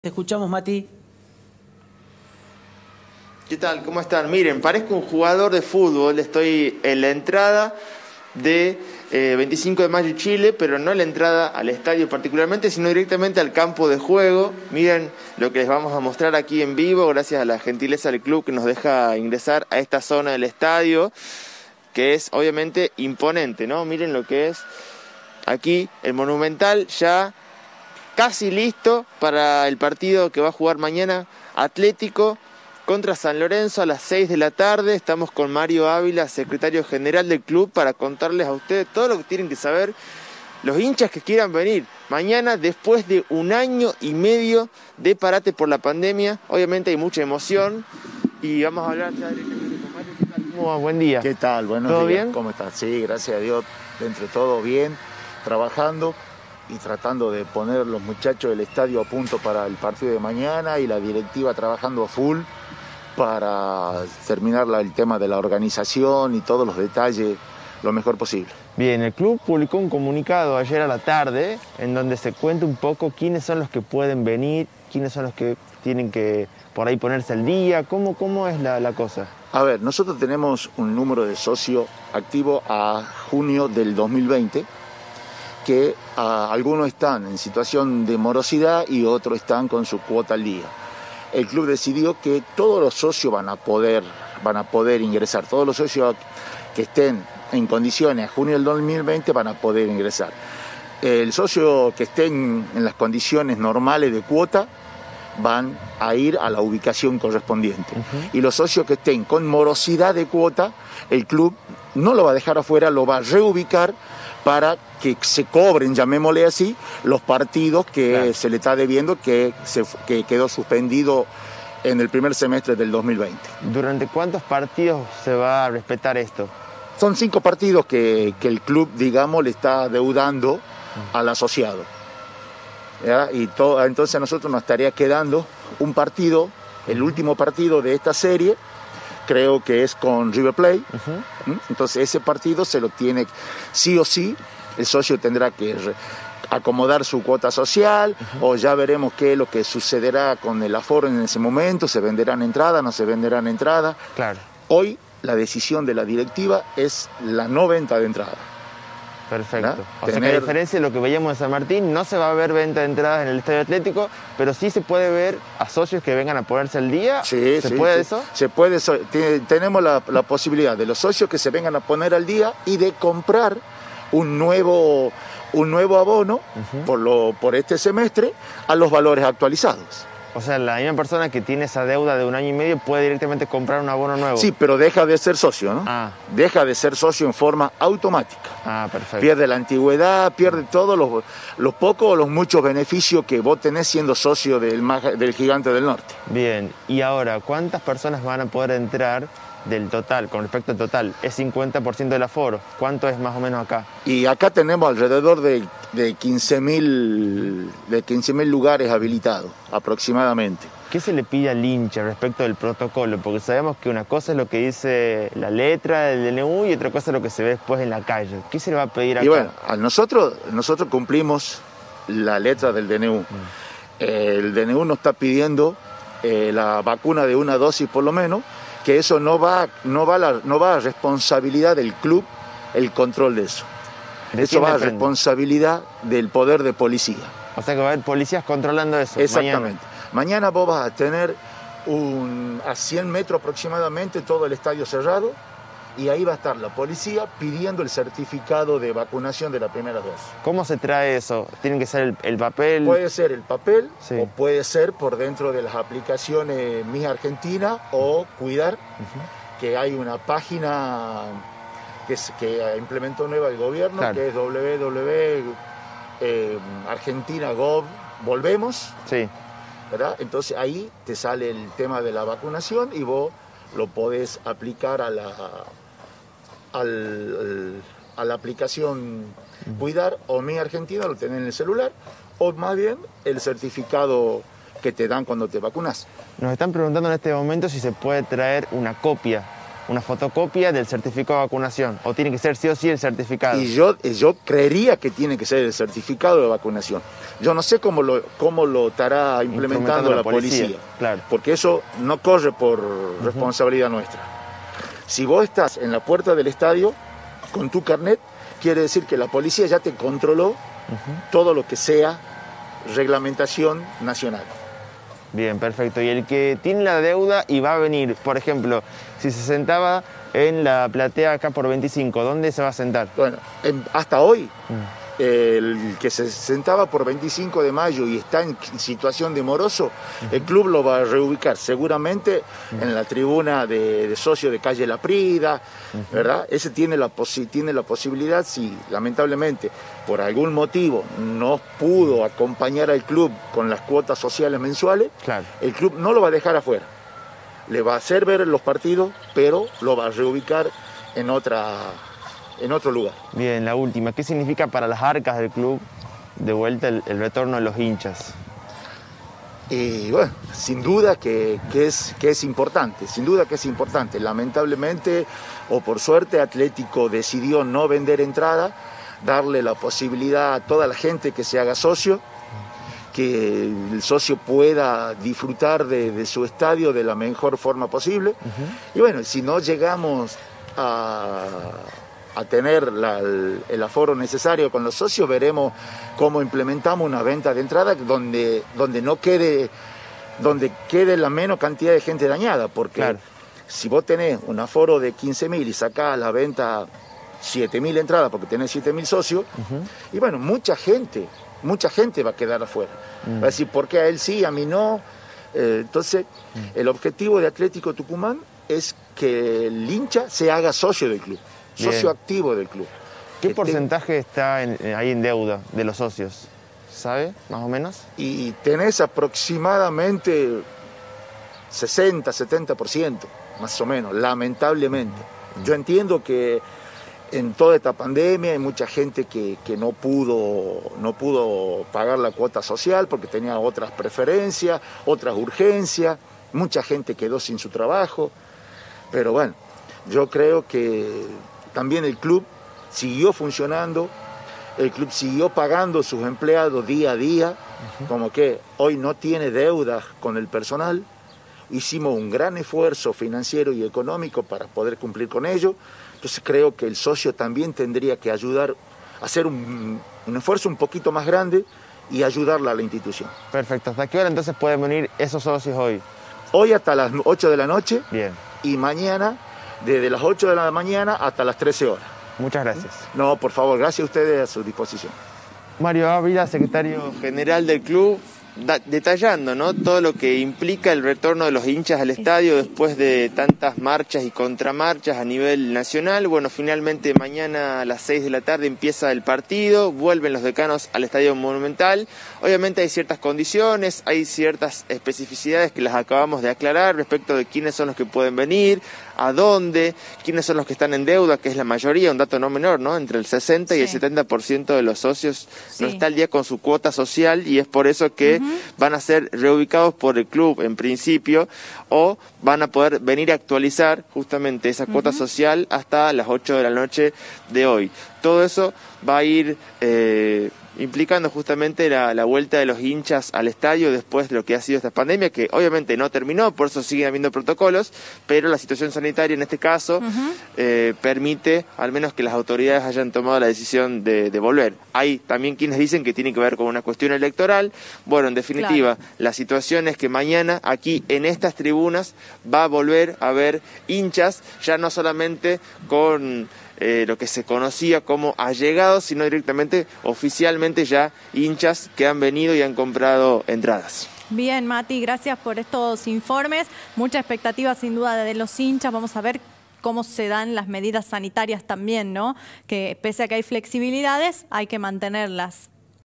Los hinchas decanos vuelven a la cancha
Entrevista a Mario Ávila, secretario de Atlético Tucumán